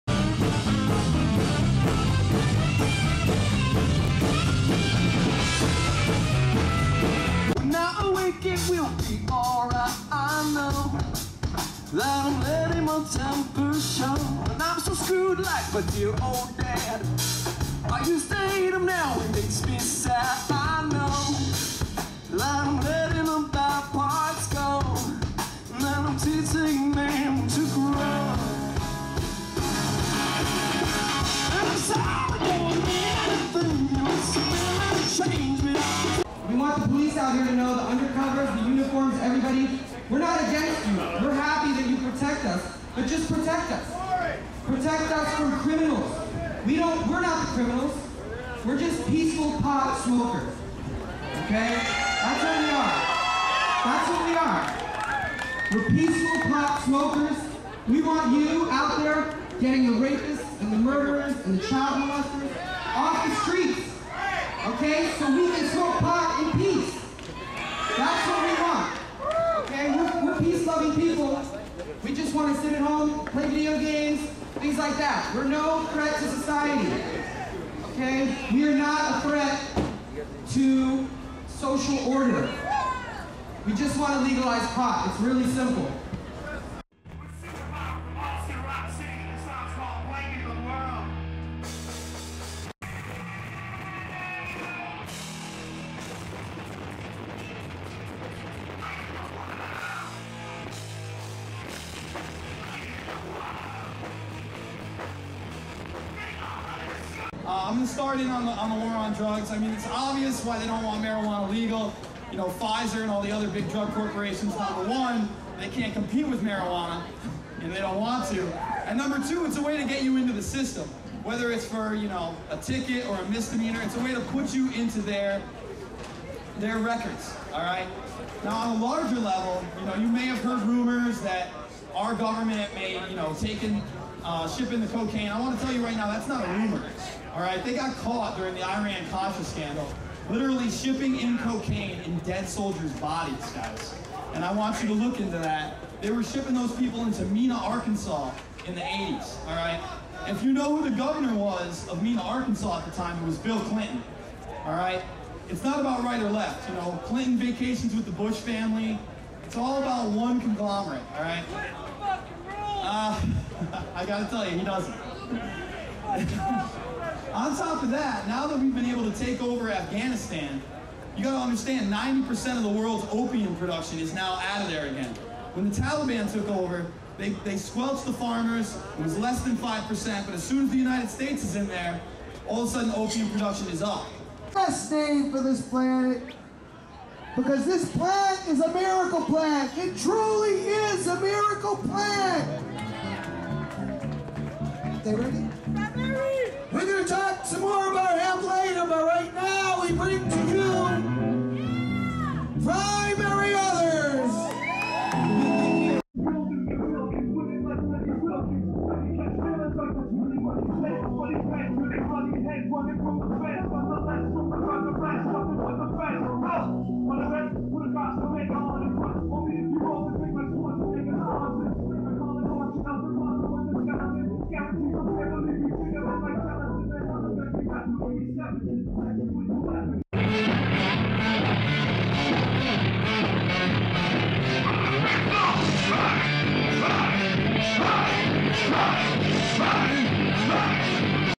2007 MASSCANN/NORML BOSTON FREEDOM RALLY- VER.3.0 HEMPFEST
Boston Freedom Rally with NORML, High Times Magazine, Graveyard BBQ, SuperPower, Primary Others, Danny Danko, Three Day Threshold, Township, Steve Epstein(MASSCANN), and Jason Bermas of Loose Change on Saturday Sept. 15th, 2007, Boston Common.

Production by Mike C, Camera Work by Scott Gacek.

Hemp, Hemp, Hooray. Boston Freedom Rally hempfest, weedfest, hemp fest, ganja rally, cannabis reform coalition.

Check out MikeCann's new company. He plans to use some of the profits to help fund marijuana reform.